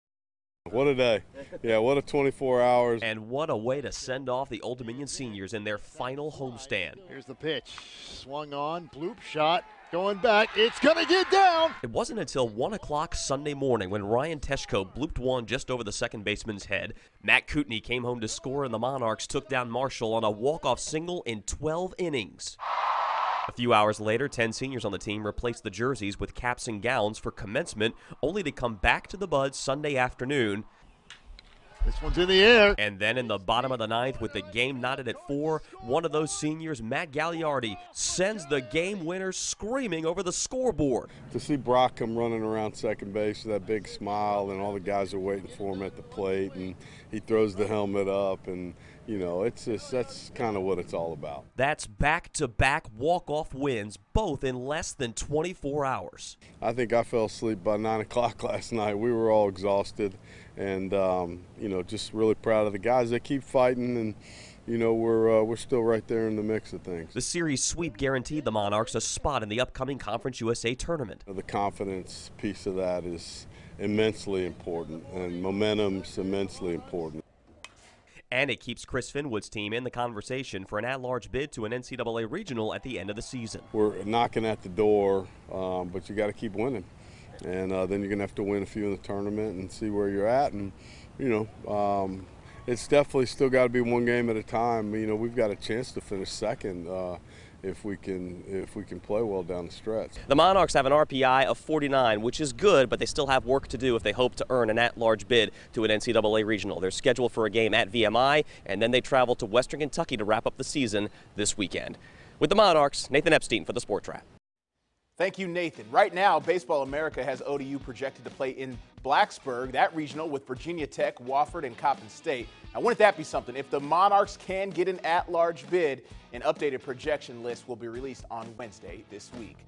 ODU earns back-to-back walk-off wins in less than 24 hours
It was a wild last home stand for the Old Dominion baseball team, which celebrated consecutive walk-off wins a little more than 12 hours apart from each other.